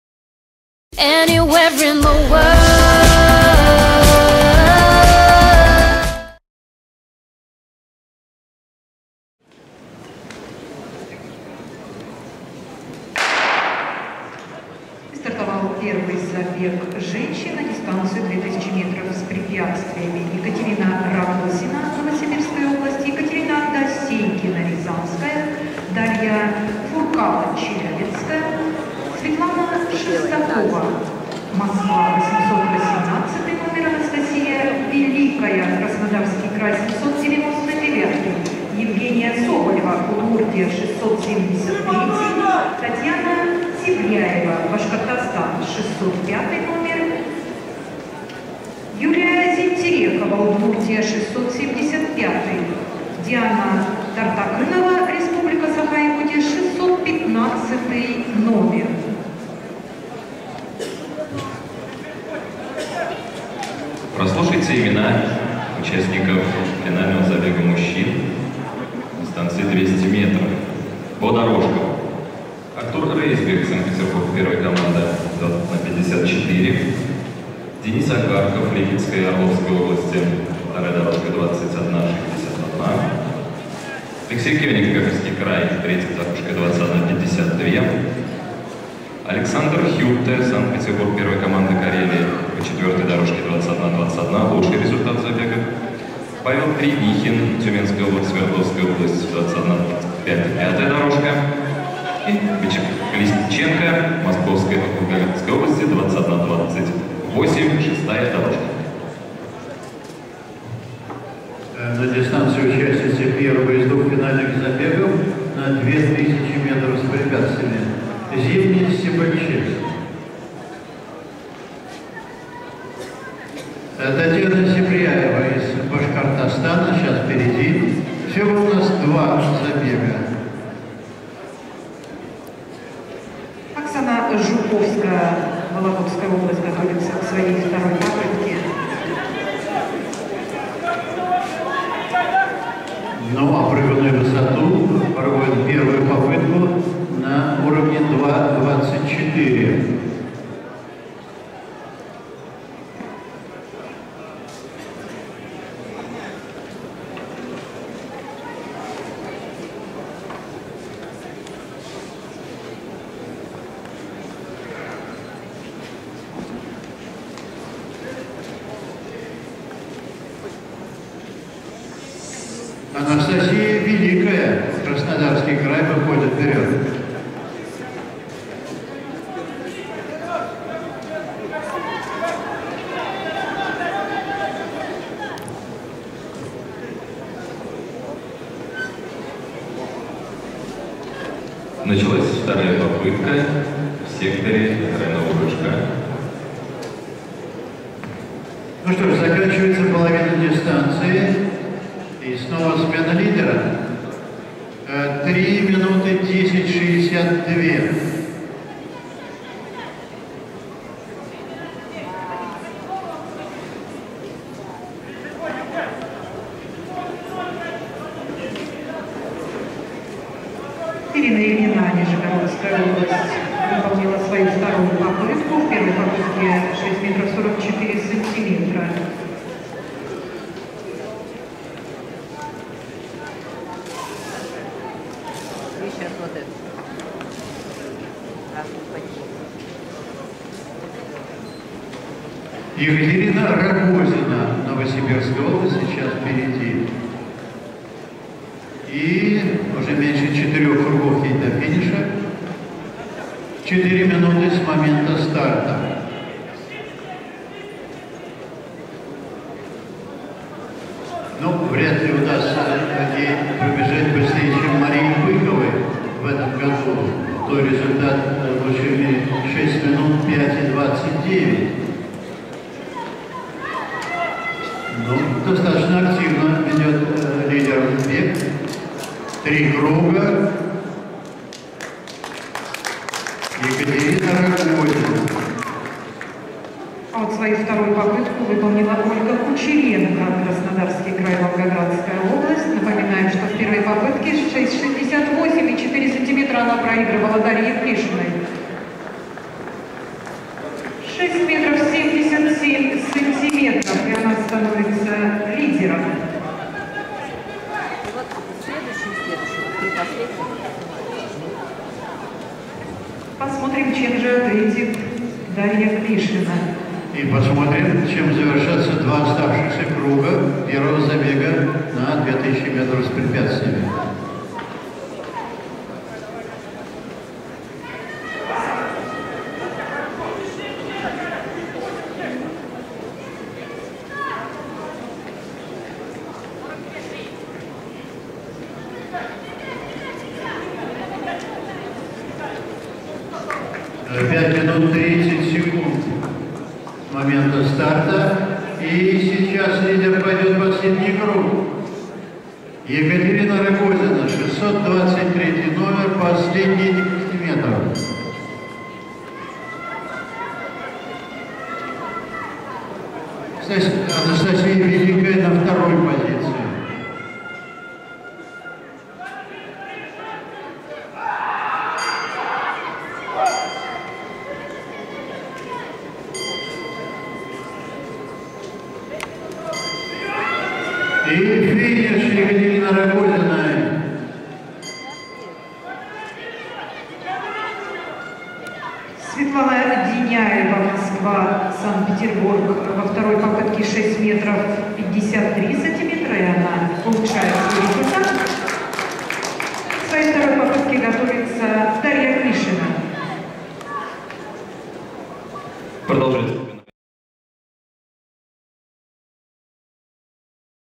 2000м с/п Женщины - 1 забег Чемпионат России 2013
2000м с/п Женщины - 1 забег Чемпионат России в помещении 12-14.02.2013